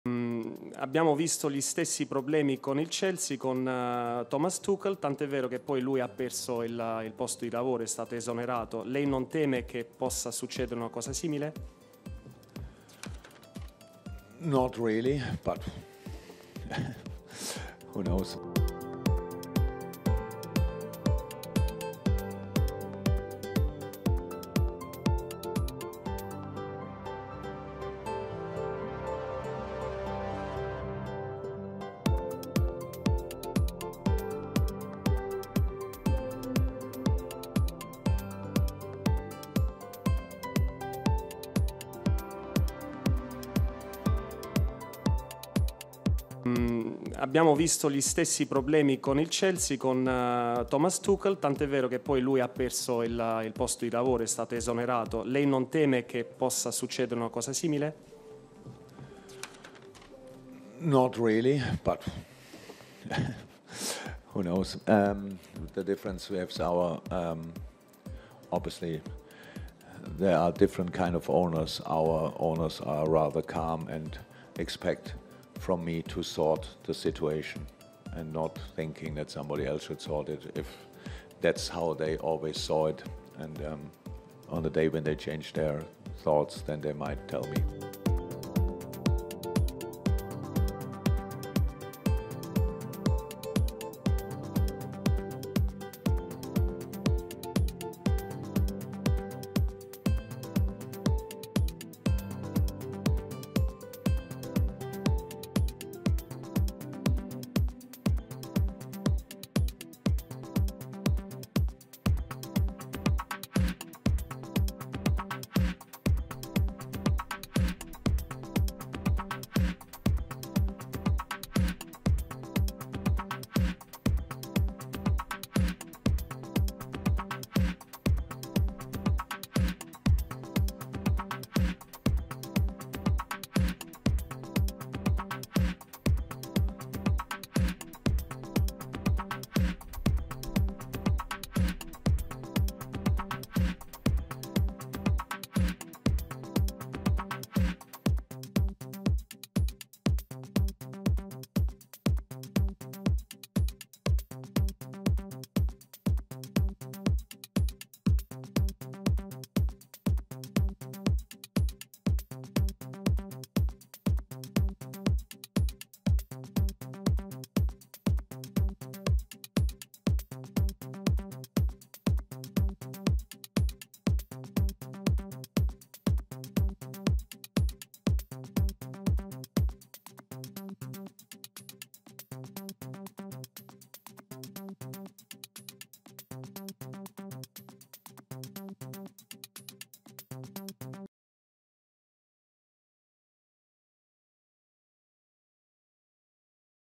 "من يعلم!".. كيف رد يورجن كلوب على احتمالية إقالته من ليفربول؟
تحدث يورجن كلوب عن مصيره مع ليفربول بعد الخسارة برباعية أمام نابولي في مجموعات تشامبيونزليج